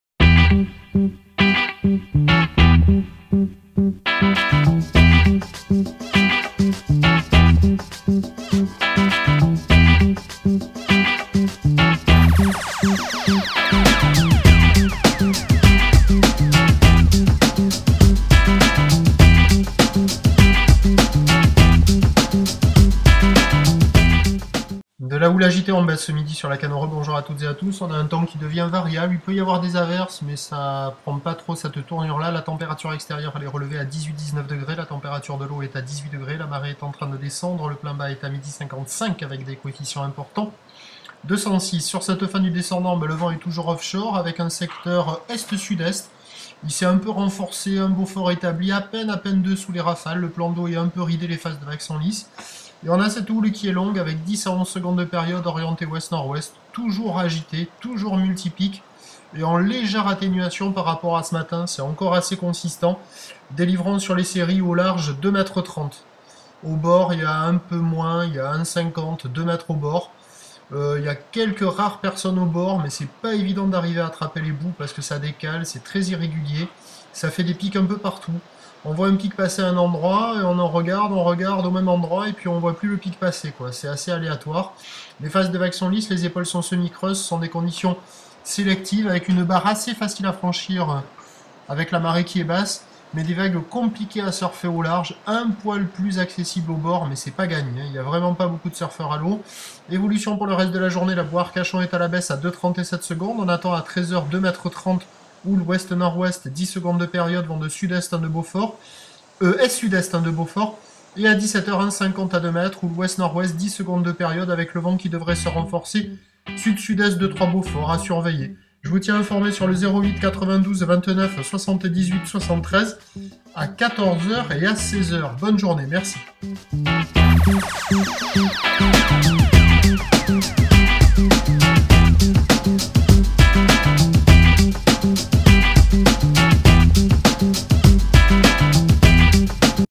Lacanau Surf Report - Vendredi 10 Octobre 11H30
Houle longue WNW en baisse, vent offshore faible à modéré.